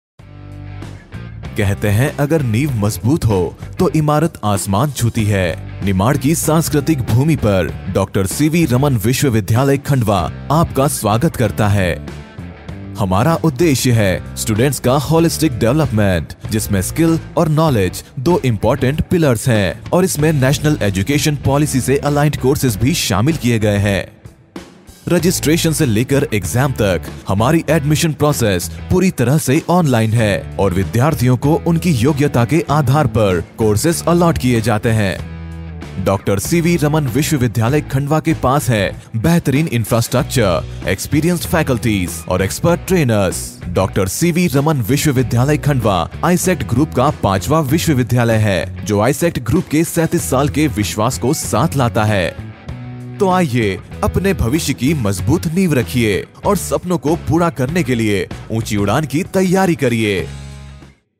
Dr. C.V. Raman University | Khandwa
Are you looking for a university that can help you unlock your full potential and achieve your dreams? Look no further than Dr. C.V.Raman University Khandwa. Our university is committed to providing a supportive and inclusive learning environment that empowers students to pursue their passions and achieve their goals.

With us you will learn about the world-class facilities and resources available at Dr. C.V. Raman University Khandwa, Get ready to be amazed by our state-of-the-art laboratories, innovative classrooms, and extensive library resources. Listen to our passionate and experienced faculty members as they share their insights and expertise in various fields. And most importantly, join a supportive community of like-minded individuals who are committed to helping each other succeed.